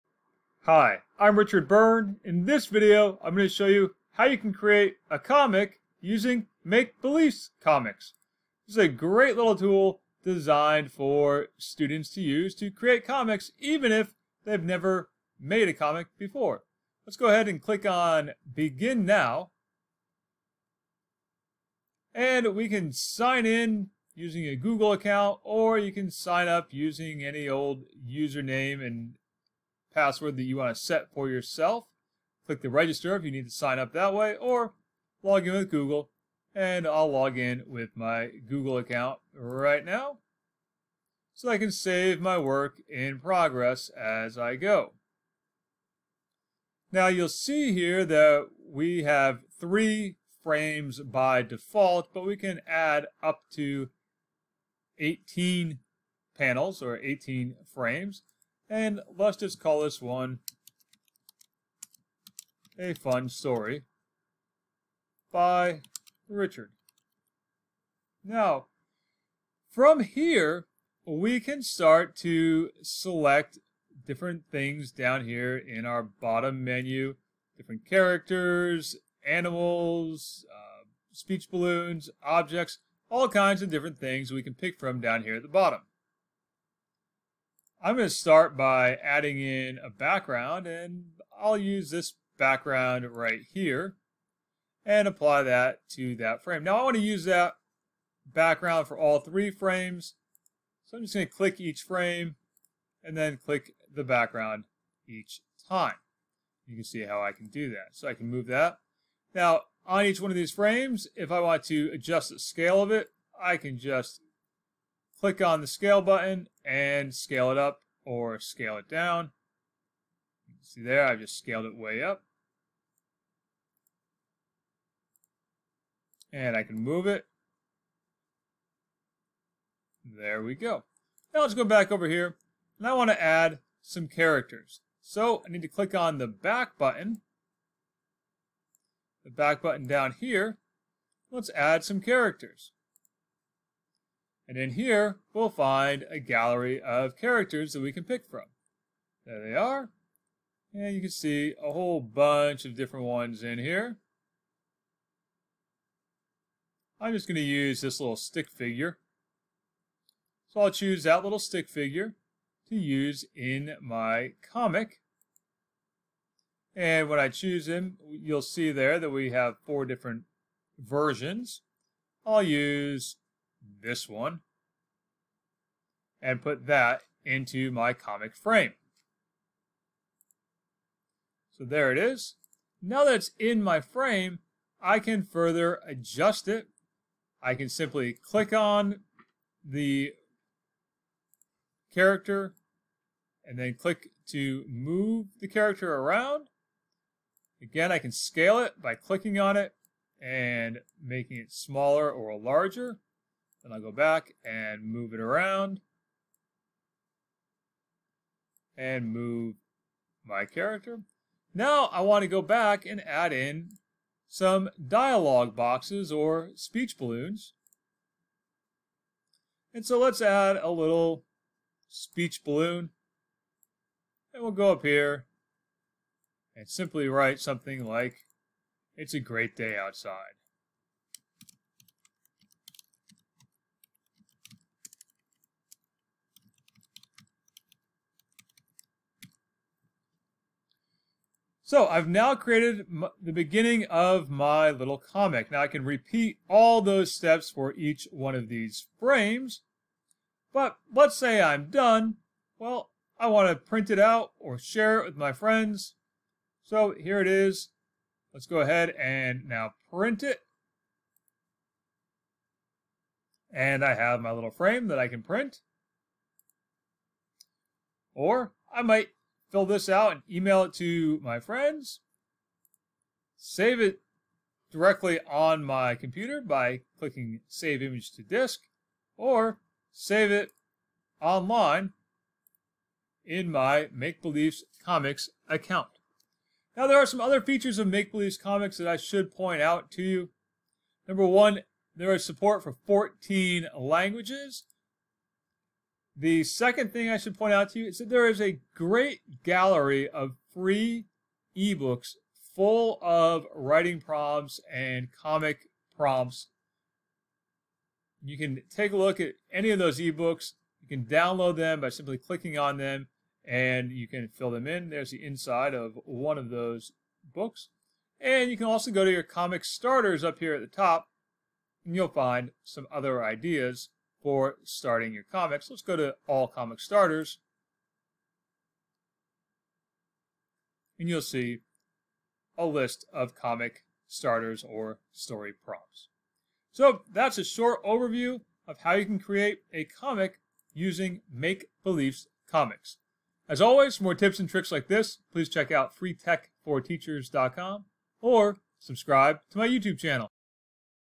Quickly Create Comics With Make Beliefs Comix
Make Beliefs Comix is a good tool for quickly creating comics from premade drawings. It's also a great source of writing prompts. Watch this video to see how it works.
🏫 Take a course with me: 🏫